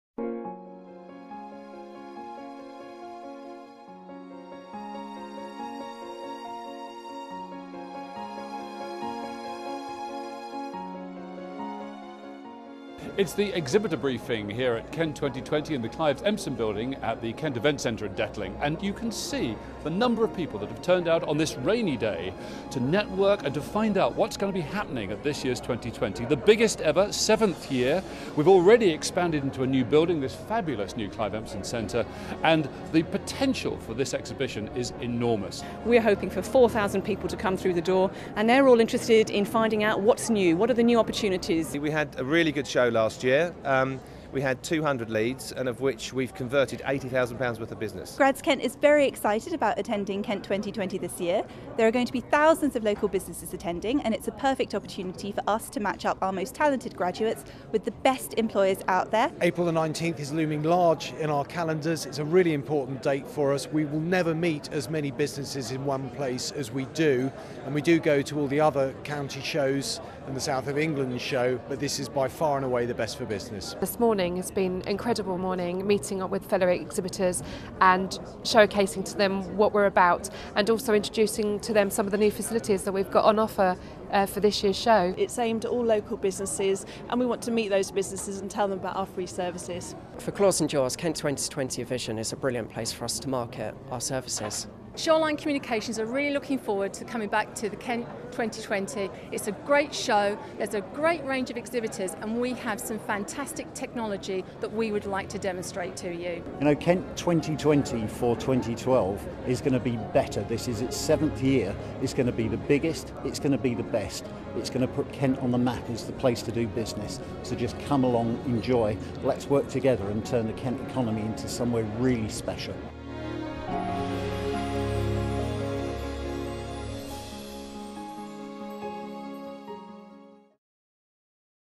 Kent 2020 2012 exhibitor pre-event.mov
Pre-event held at the Kent Event Centre on 26th January 2012 to provide support and information to confirmed and prospect exhibitors, ahead of the main exhibition and conference on April 19th 2012